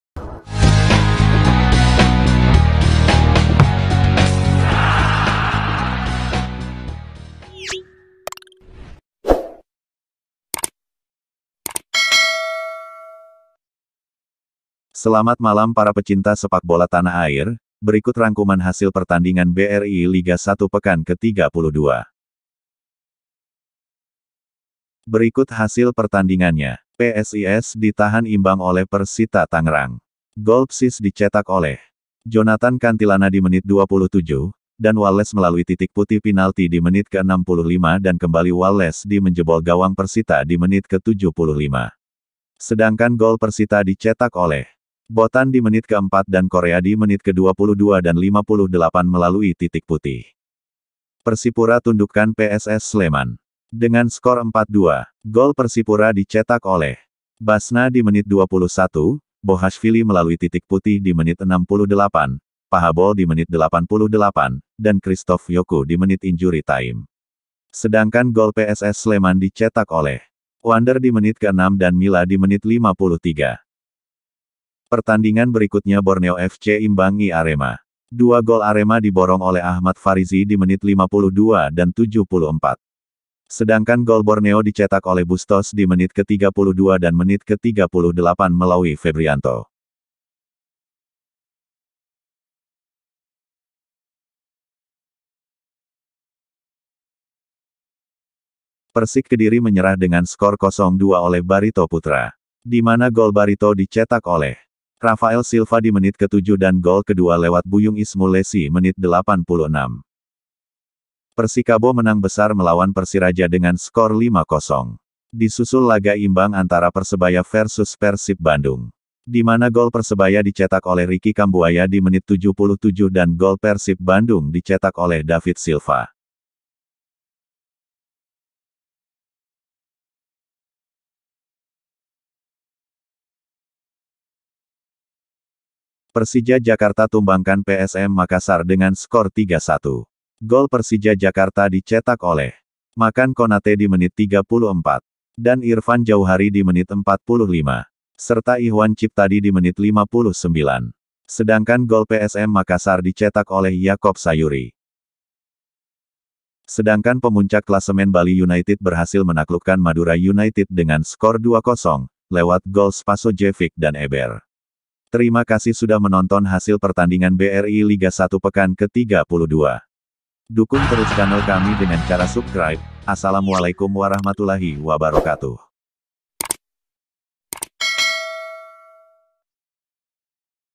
Rangkuman lengkap hasil pertandingan BRI liga 1 || Pekan ke 32|| Berita hari ini
Hasil pertandingan BRI liga 1 pekan ke 32

Persija,Persita,PSS Sleman,Bali united,arema, Persib, Persebaya, Persipura,PSM Makassar, PSIS Semarang, Madura united,Persiraja, Persikabo